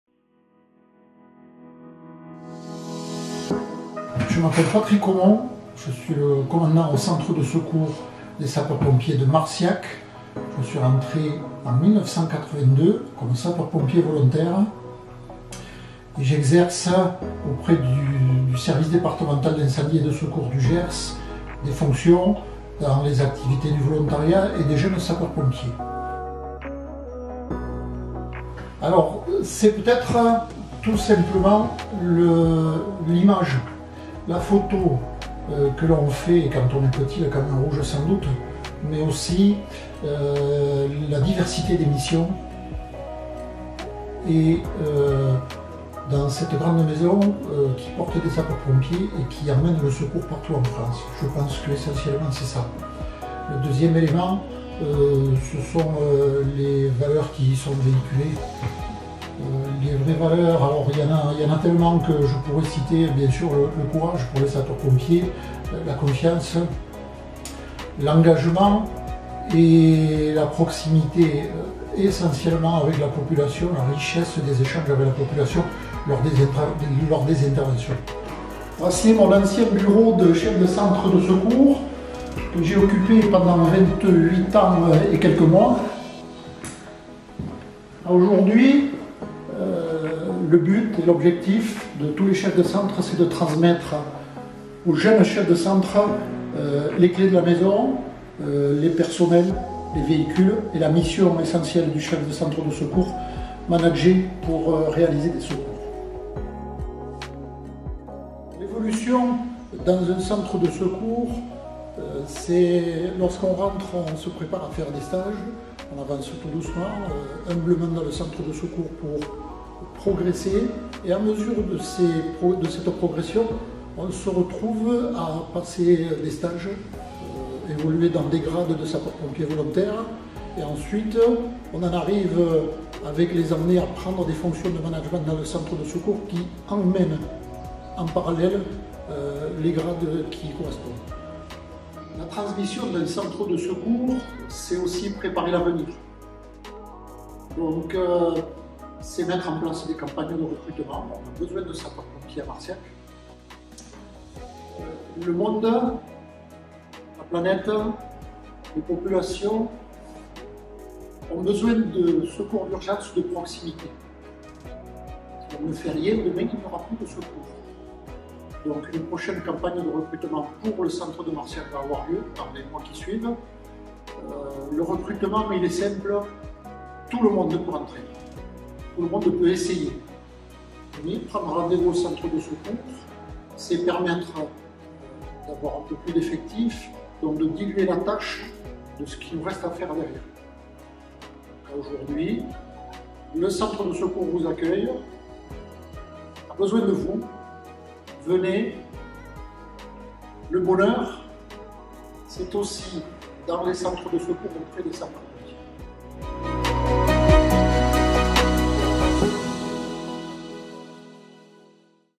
VOLONTARIAT 👨‍🚒 RENCONTRE AVEC LE COMMANDANT PATRICK CAUMONT
👨‍🚒 Le Commandant Patrick Caumont s'est engagé au centre d’incendie et de secours de Marciac en 1982 à l'âge de 22 ans. Pendant plus de 28 ans, il a occupé le poste de chef de centre et aujourd'hui il est adjoint au chef du groupement territorial Sud.

🚒 Le centre d'incendie et de secours de Marciac a besoin de sapeurs-pompiers volontaires. Tout le monde a sa place parmi les pompiers volontaires et tout le monde peut apporter sa pierre à l’édifice. Engagez-vous ! 🫵